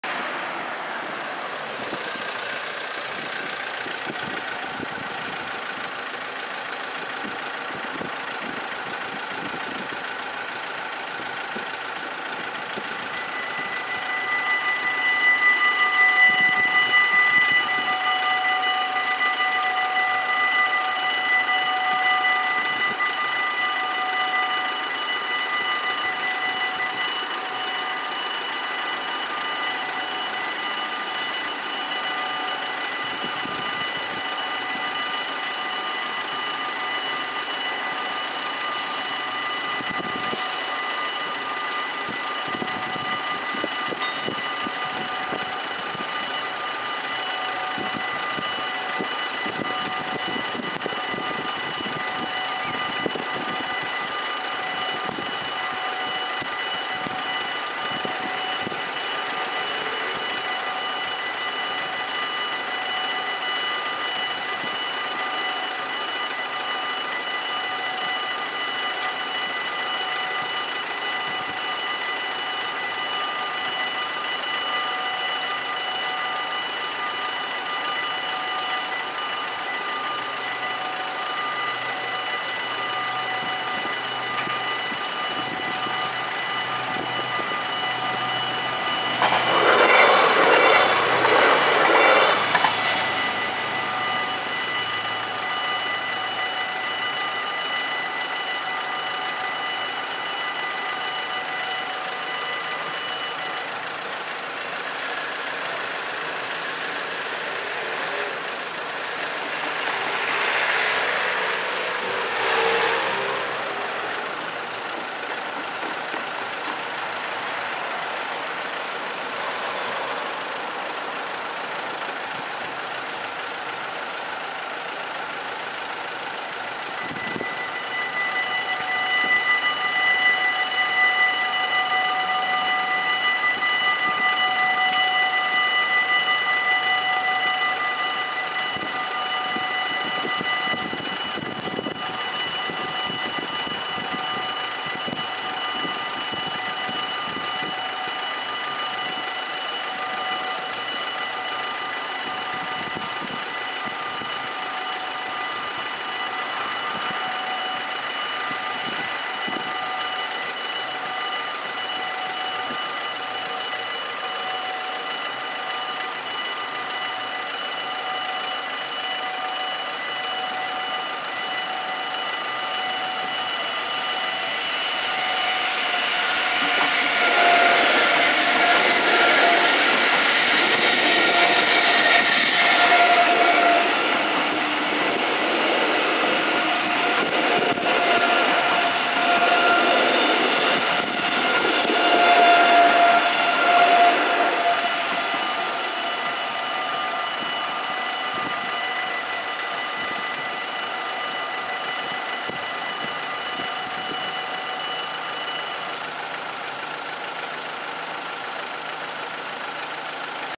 Spoorwegovergang Zevenaar // Dutch railroad crossing
Locatie: Molenstraat
Traject: Arnhem - Doetinchem/Winterswijk, Arnhem - Emmerich
Soort: AHOB
Rode lichten: 8
Bellen: 4
Bomen: 2
Andreaskruisen: 2
Passeren:
- GTW-D richting Arnhem
- ICE richting Frankfurt Hbf